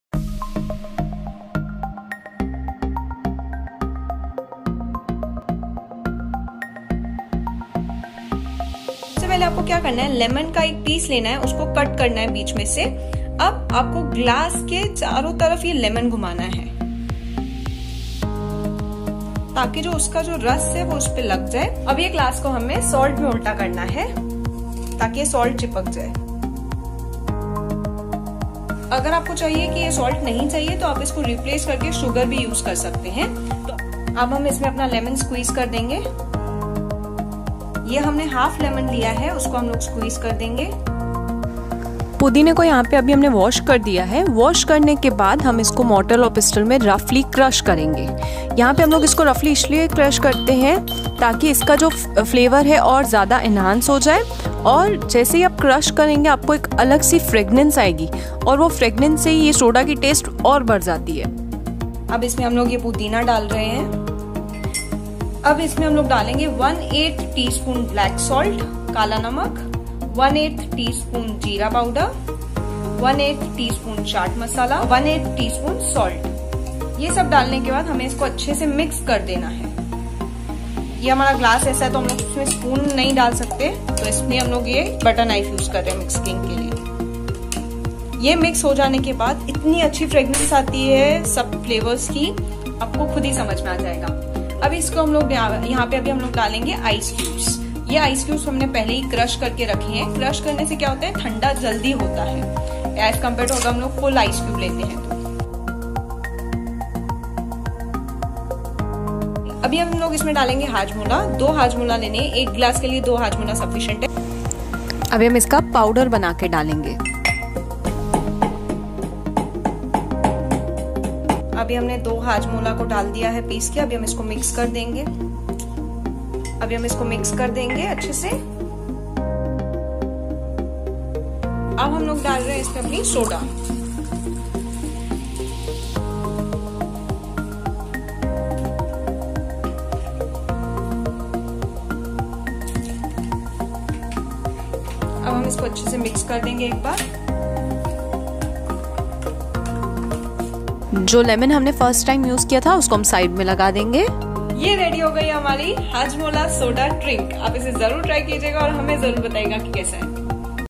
Hajmola Lemon Drink l Hajmola Soda l Summer Refreshing Drink
Hey Everyone
Today we have made a summer refreshing drink using Hajmola. This is very effective for digestion. Use only 2 Hajmolas in 1 glass.